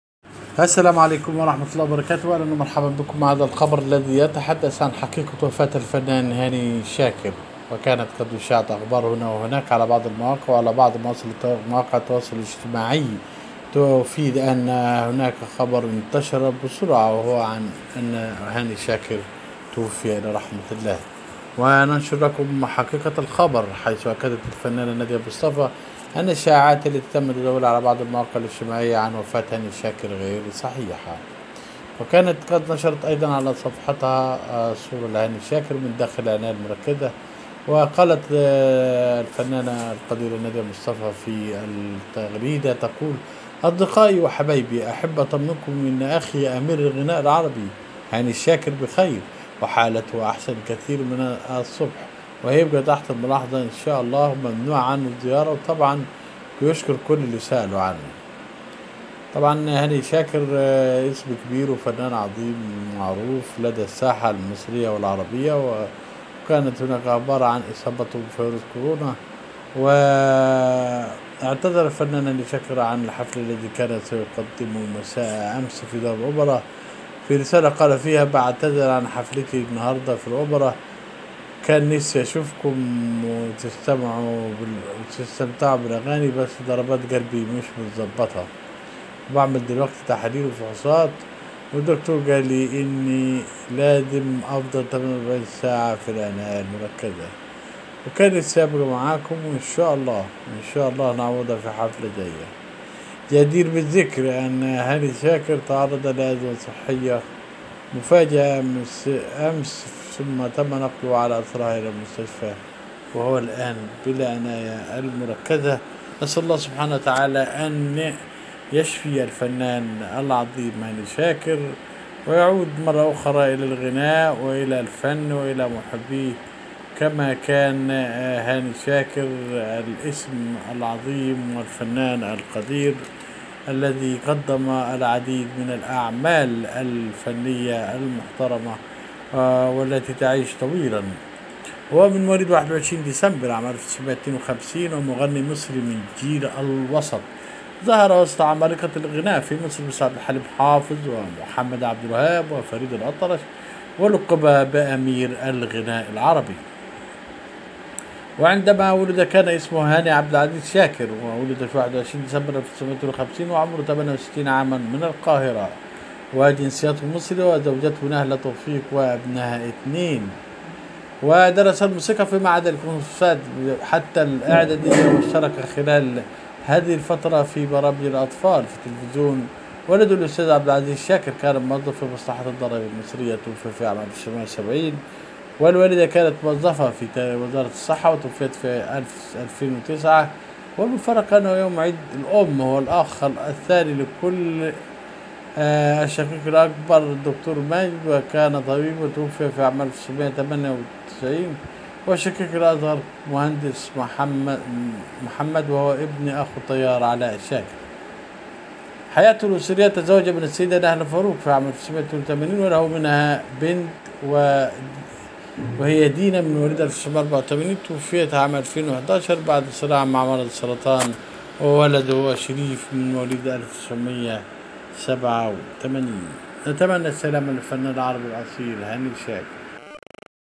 وفاة هاني شاكر خبر كاذب لكن الفنان في العناية المركزة وينتظر دعواتكم
حقيقة خبر وفاة هاني شاكر ليس لها اساس من الصحة لكن لا احد يمنع ان الفنان مريض وبالعناية المركزة استمع الي هذا التقرير عن حالة الفنان القدير .
وفاة هاني شاكر،
من هو هاني شاكر،
نوع مرض هاني شاكر